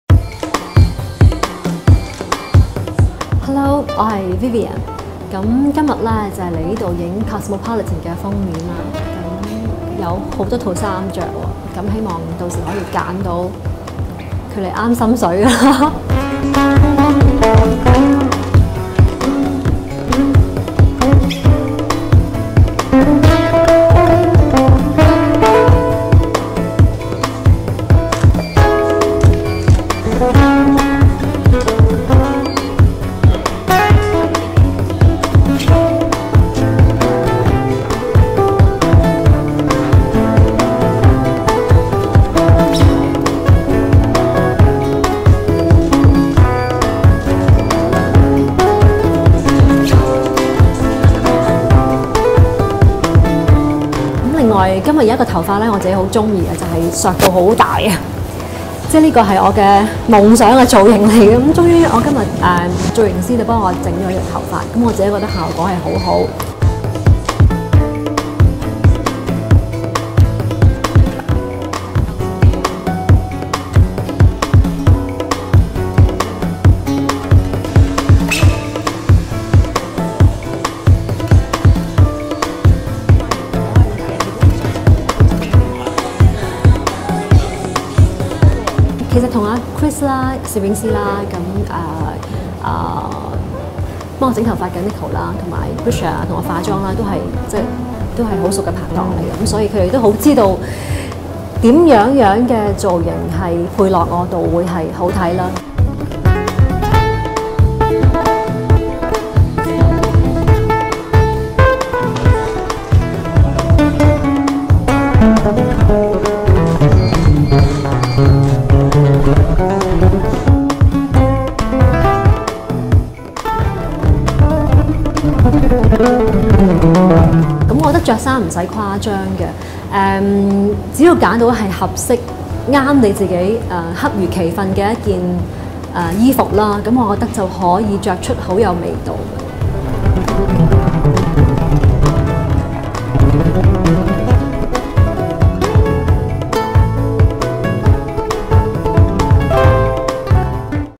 Vivian Chow 周慧敏 on October's Cosmopolitan (幕後花絮)
For those familiar with Hong Kong's golden age of Cantopop, Vivian Chow Wai-man was considered one of the top divas of her generation. Having retired from the limelight for nearly 20 years, Chow does regular charity work behind the scenes and is a staunch animal activist.

Cosmopolitan magazine invited Chow to be on their cover for October's Fall issue and we take a look behind-the-scenes at the photo shoot.

Chow also discusses her taste in fashion and what to expect in her Winter collection.

周慧敏: 幕後花絮

周慧敏是香港流行曲黃金年代的玉女掌門人，退出娛樂圈至今已近二十年，期間一直致力於慈善工作，並熱心宣揚愛護動物訊息。

《Cosmopolitan》十月號誠意邀得周慧敏擔任封面人物。本視頻將送上封面拍攝花絮。

周慧敏還會暢談時裝心得，以及冬季時興新裝。